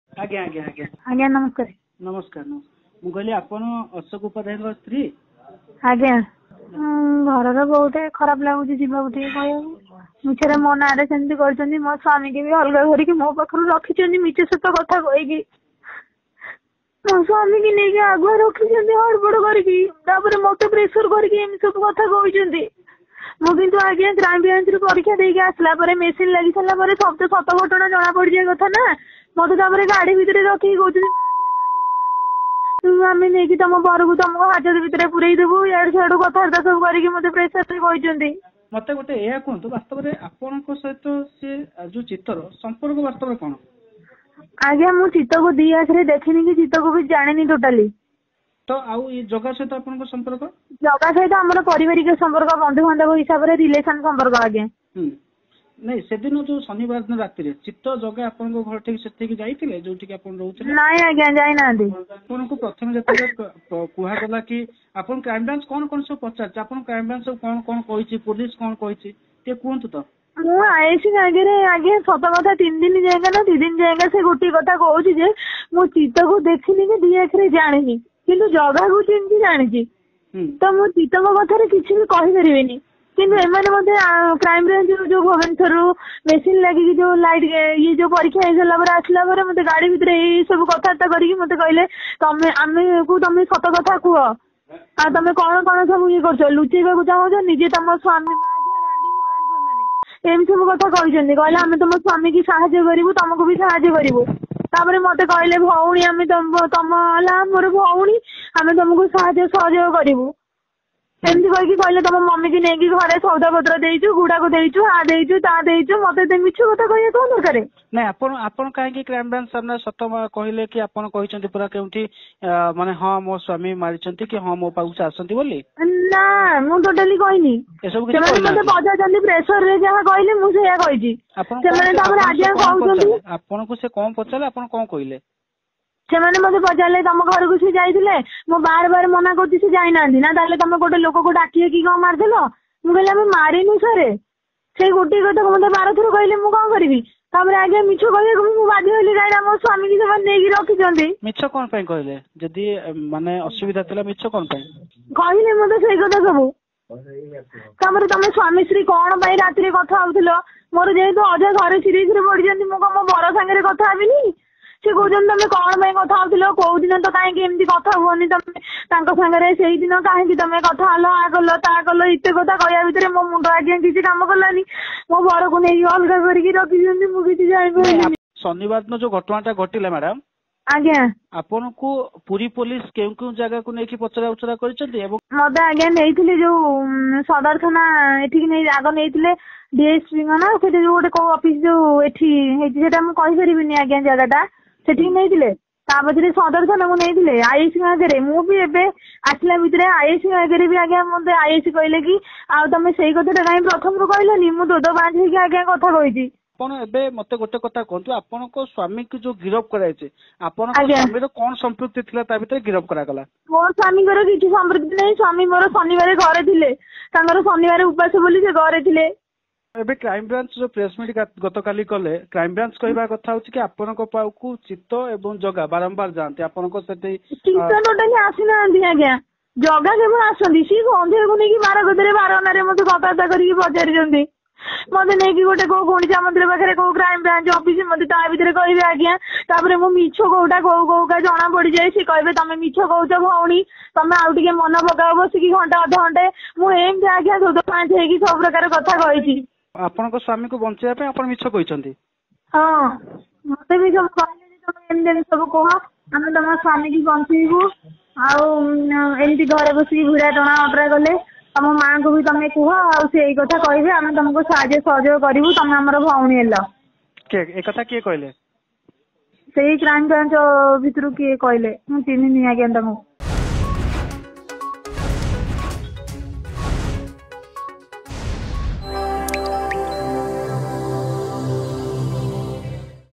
ଗିରଫ ଅଶୋକ ଉପାଧ୍ୟାୟଙ୍କ ପତ୍ନୀ ଆଣିଲେ ସାଂଘାତିକ ଅଭିଯୋଗ
ଚିତ୍ତ ମର୍ଡର ମିଷ୍ଟ୍ରିରେ ନୂଆ ଟ୍ବିଷ୍ଟ । ଗିରଫ ଅଶୋକ ଉପାଧ୍ୟାୟଙ୍କ ପତ୍ନୀ ଆଣିଲେ ସାଂଘାତିକ ଅଭିଯୋଗ । କହିଲେ ଆମ ଉପରେ ଚାପ ପକାଇଛି କ୍ରାଇମବ୍ରାଞ୍ଚ । ଡରାଇ ଧମକାଇ ମିଛରେ ଫଶାଇଛି କ୍ରାଇମବ୍ରାଞ୍ଚ । ଶୁଣନ୍ତୁ ଅଶୋକଙ୍କ ପତ୍ନୀଙ୍କ ଅଡିଓ ।